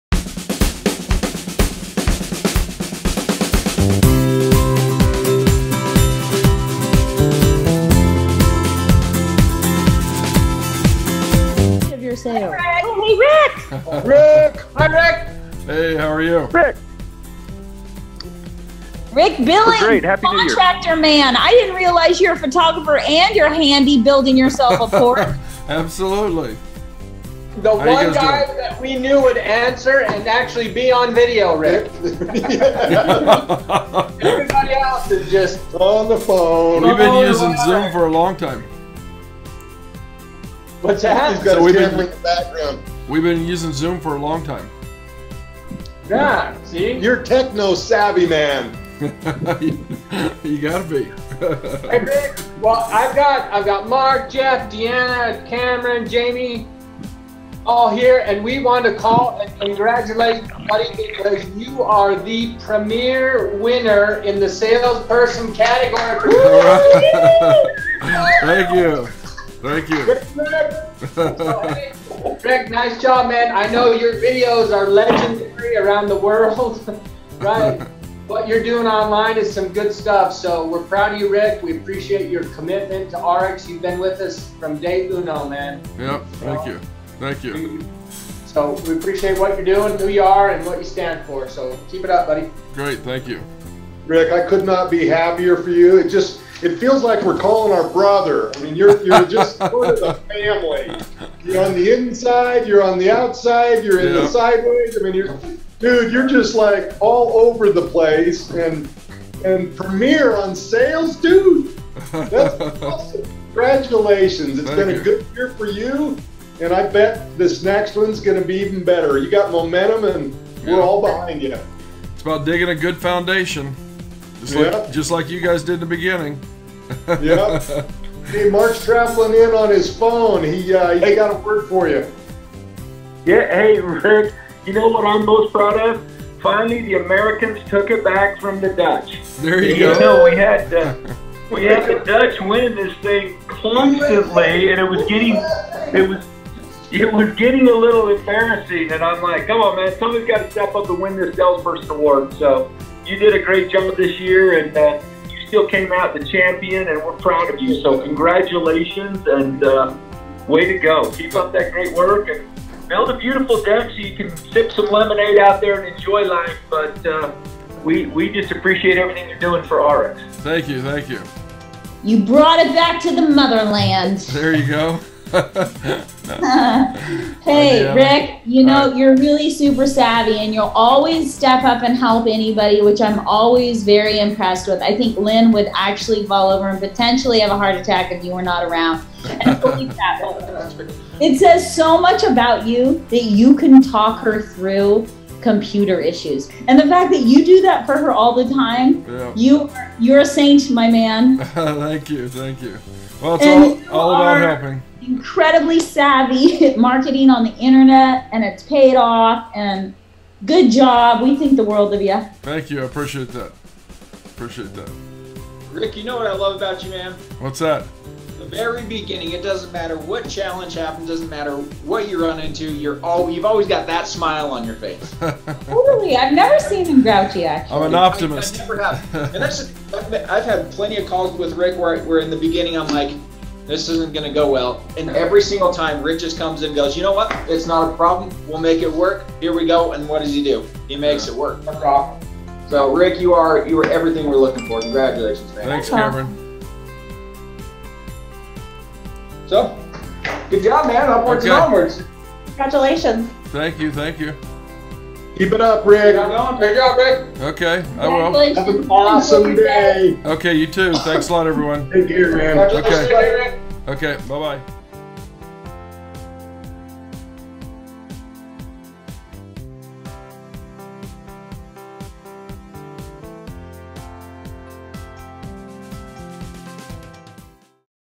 The Owners of ARIIX call Rick Billings - 2016 Premier Salesperson
The Owners of ARIIX called Rick Billings and congratulated him for the  2016 Premier Salesperson. The award goes to the person in North America that has generated the most points from Preferred Customers.